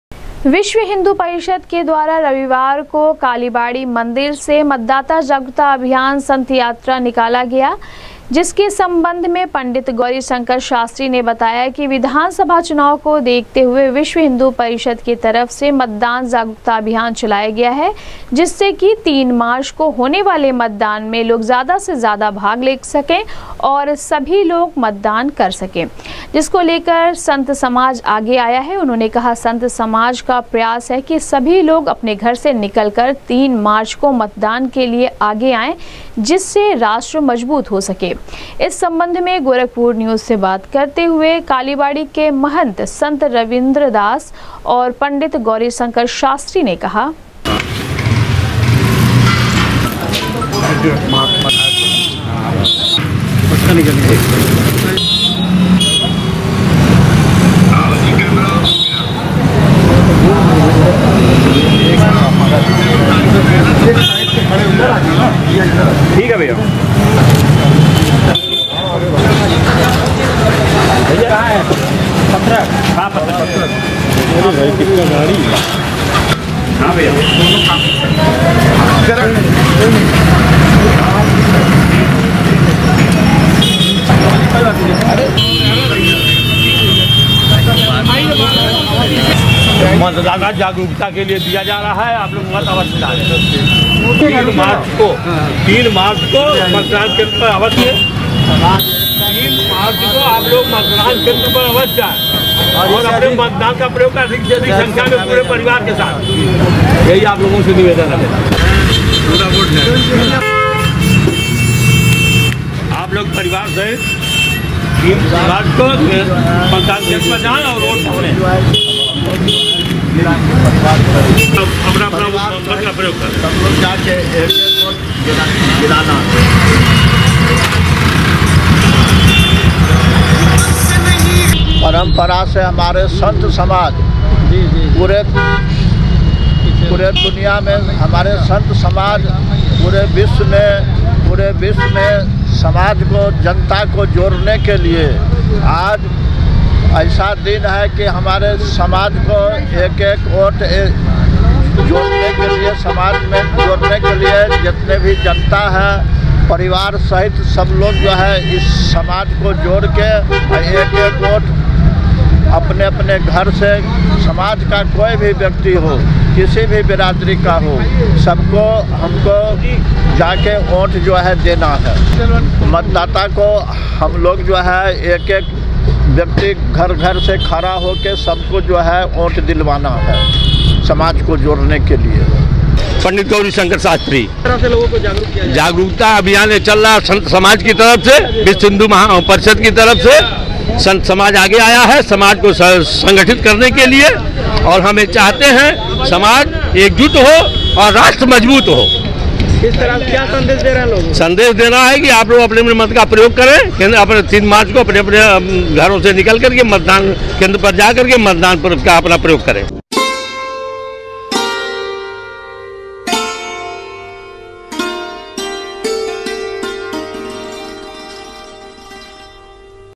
विश्व हिंदू परिषद के तत्वावधान में काली बाड़ी मंदिर से निकाला गया मतदाता जागरूकता अभियान संत यात्रा
विश्व हिंदू परिषद के तत्वावधान में काली बाड़ी मंदिर से निकाला गया मतदाता जागरूकता अभियान संत यात्रा ,लोगों को तीन मार्च को होने वाले मतदान के लिए किया गया जागरूक